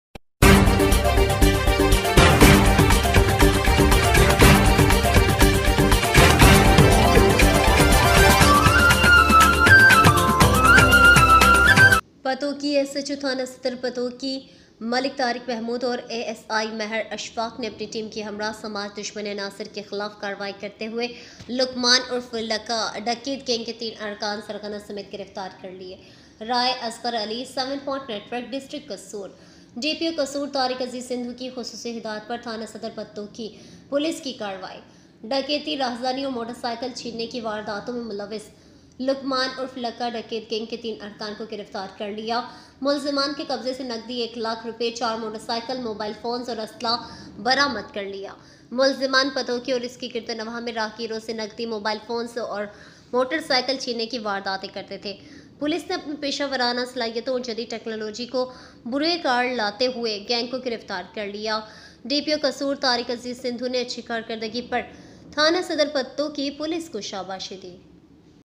Police operation of Sadar Patu police station,arrested three members of Luqman alias Lakh Dakit gang
Patuki SHO Police Station President Patuki Malik Tariq Mehmood and ASI Mehr Ashfaq along with their team arrested three members of Luqman alias Laqha dacoit gang along with the ringleaders, DPO Kasur Tariq Aziz Sindhu  On the instructions of Saddar Patuki police station, three members of Luqman aka Laqha dacoit gang involved in robbery, robbery and snatching of motorcycles were arrested and Rs 100,000 in cash, 4 motorcycles, mobile phones and weapons were recovered from the possession of the accused.  The accused used to steal cash, mobile phones and motorcycles from passers-by in Patuki and its surroundings.  The police arrested the gang using their professional skills and modern technology.7point network.03008184643.